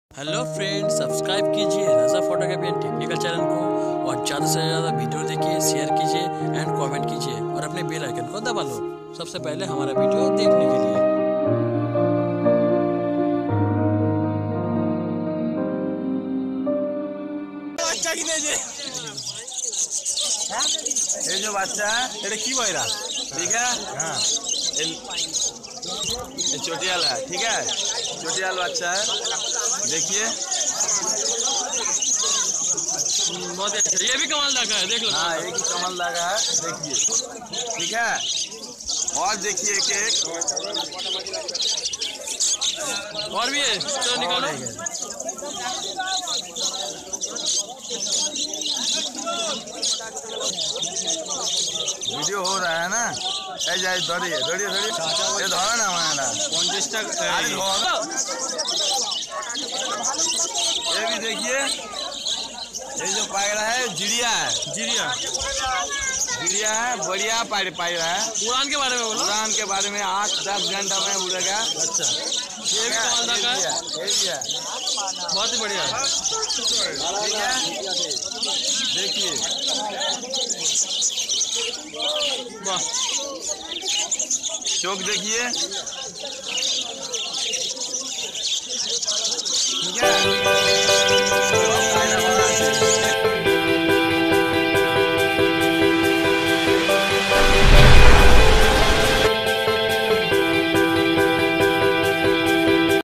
Madrasi chotti dar & Madrasi pigeon "kamal da" ka by Raza Photography & Technical
Baria Kabutar hai Kamal Da ka.. suddenly meet in Uluberia market ...and small Introduction Kamal pigeon
Very high Flying pigeon by
Raza Photography & Technical